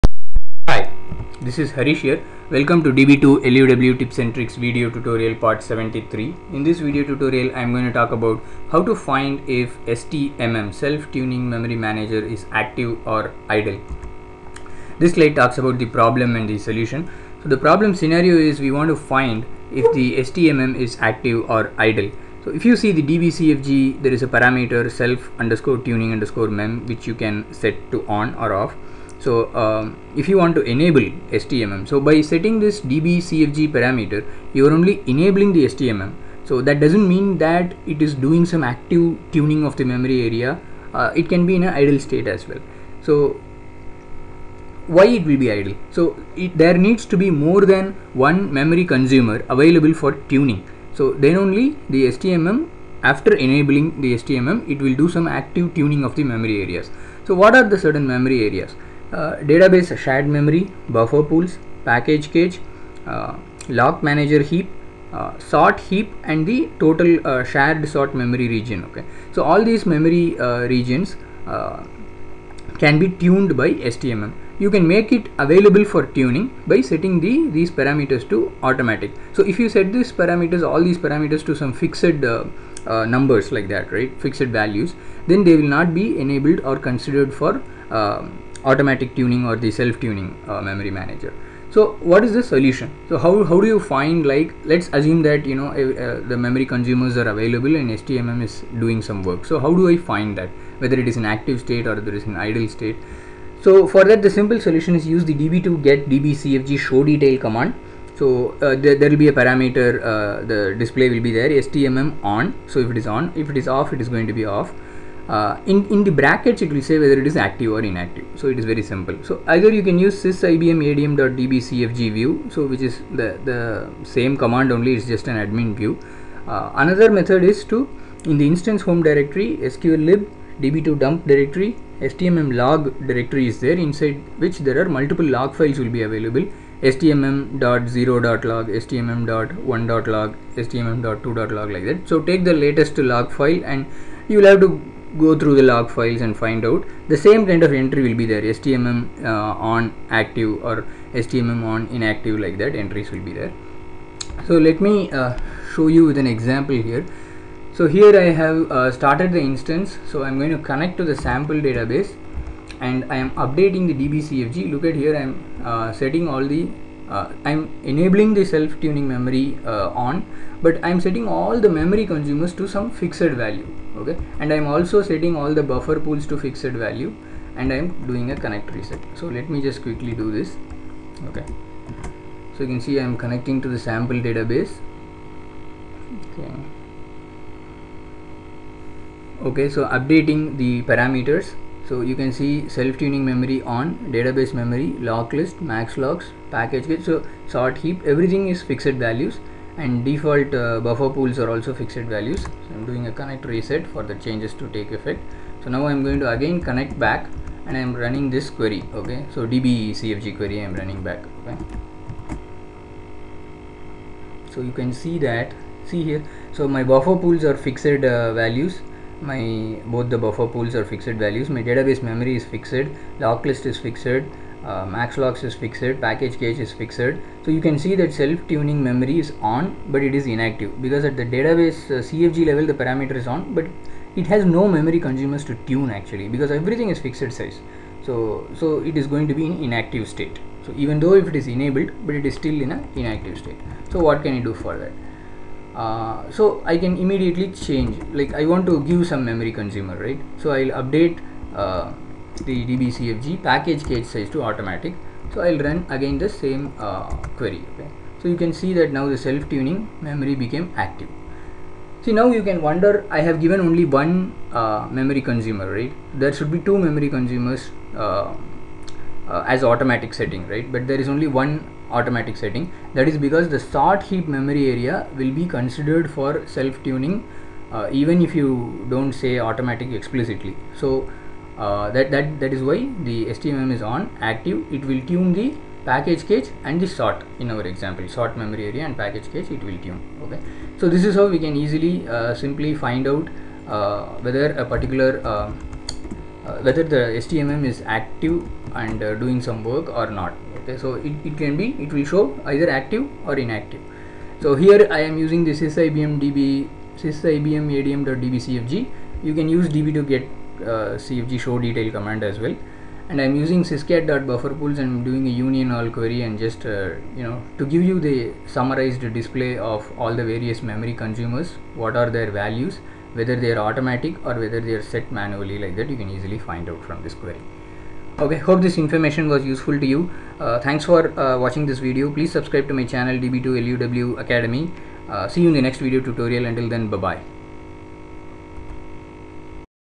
DB2 Tips n Tricks Part 73 - How to Find if STMM is Active or Idle
How to Find if STMM is Active or Idle
Solution:
db2 get db cfg show detail
SYSIBMADM.DBCFG
Instance_Home/sqllib/db2dump/stmmlog/stmm.n.log

Happy Learning & Sharing